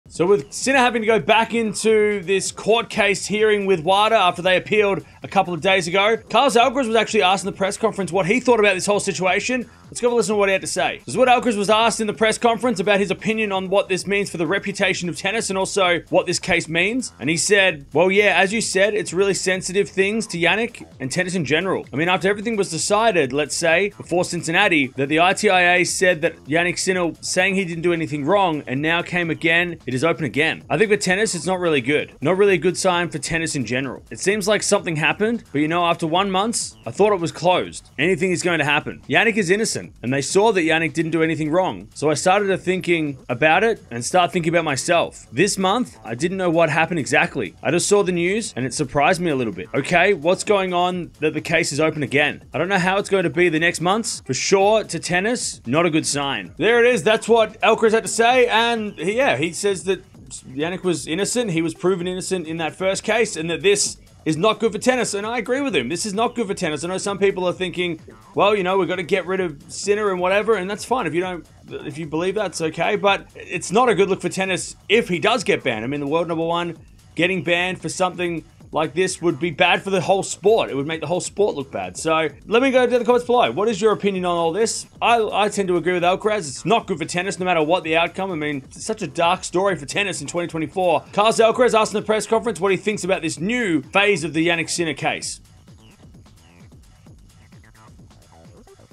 Alcaraz Responds to WADA Appeal over Sinner Failed Drug Tests | Tennis News
Carlos Alcaraz has responded in his post match press conference to the WADA appeal in the Sinner Dooping Saga. Alcaraz was suprised that their was a appeal and will have to play under the cloud of a potential ban again thie season. The CAS will make a decision in the coming months whether or not WADA's appeal of banning Sinner between 1-2 years will be granted.

0:00 | Intro
0:10 | Alcaraz Response
0:56 | Outro